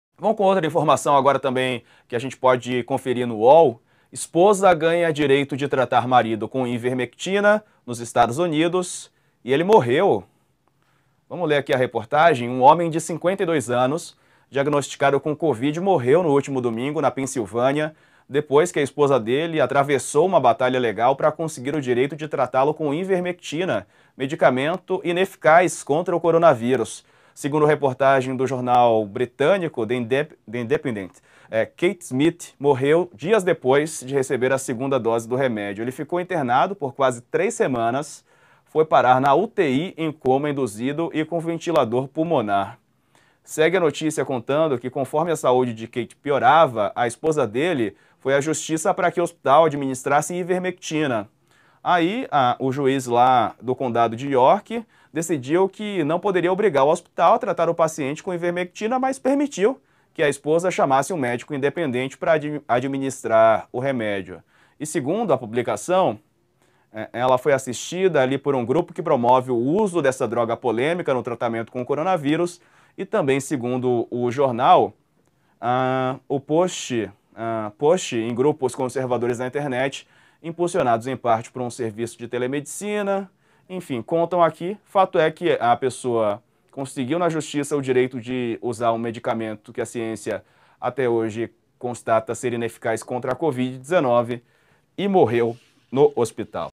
Esposa ganha direito de tratar marido com ivermectina nos EUA e homem morre
Um homem de 52 anos diagnosticado com covid-19 morreu no último domingo (12), na Pensilvânia (EUA), após sua esposa ter travado uma batalha legal para conseguir o direito de tratá-lo com ivermectina, medicamento sem comprovação contra o coronavírus